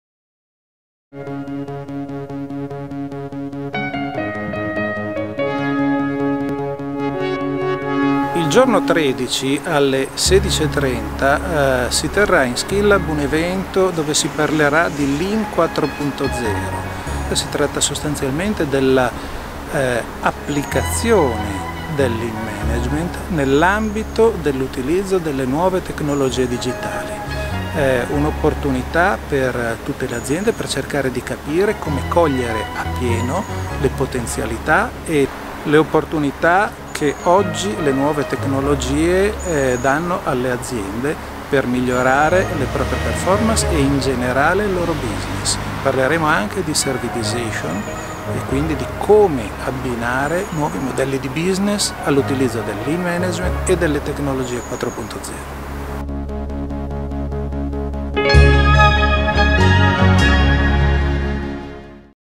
Paolo Sganzerla: Lean 4.0
I principi del Lean Thinking coniugati con l’impiego delle tecnologie digitali rappresentano il percorso da intraprendere per poter cogliere appieno le opportunità offerte dalla 4^ Rivoluzione Industriale. Dalla loro combinazione, infatti, si possono aprire nuove opportunità per le imprese dal generare nuovo valore per il cliente, alla vendita di nuovi servizi abbinati ai prodotti già presenti in azienda.